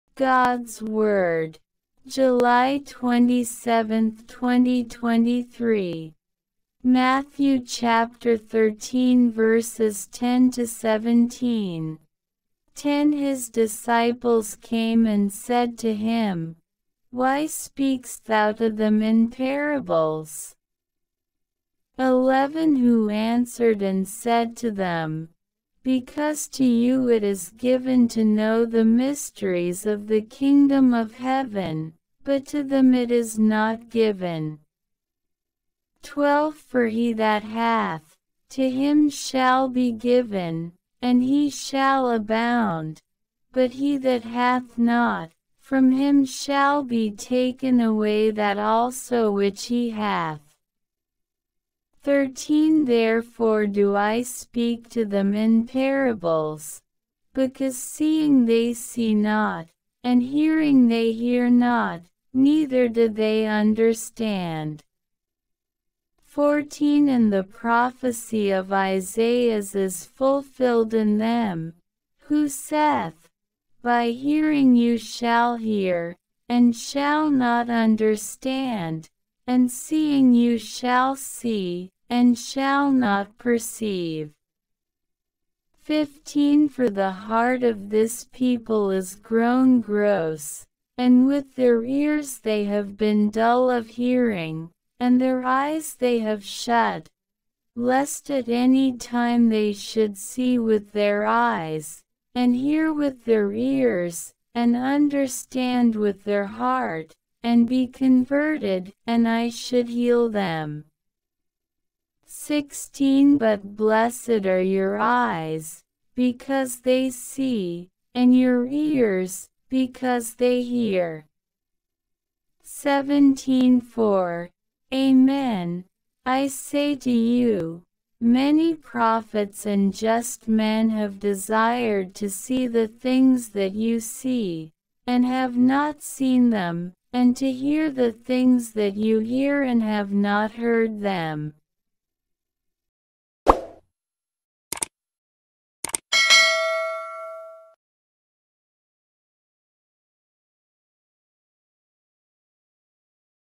Daily Bible Verse July 27, 2023 Matthew 13: 10-17 God's Word Bible Reading
10 His disciples came and said to him: Why speakest thou to them in parables?

11 Who answered and said to them: Because to you it is given to know the mysteries of the kingdom of heaven: but to them it is not given.

12 For he that hath, to him shall be given, and he shall abound: but he that hath not, from him shall be taken away that also which he hath.

13 Therefore do I speak to them in parables: because seeing they see not, and hearing they hear not, neither do they understand.

14 And the prophecy of Isaias is fulfilled in them, who saith: By hearing you shall hear, and shall not understand: and seeing you shall see, and shall not perceive.

15 For the heart of this people is grown gross, and with their ears they have been dull of hearing, and their eyes they have shut: lest at any time they should see with their eyes, and hear with their ears, and understand with their heart, and be converted, and I should heal them.

16 But blessed are your eyes, because they see, and your ears, because they hear.

17 For, amen, I say to you, many prophets and just men have desired to see the things that you see, and have not seen them, and to hear the things that you hear and have not heard them.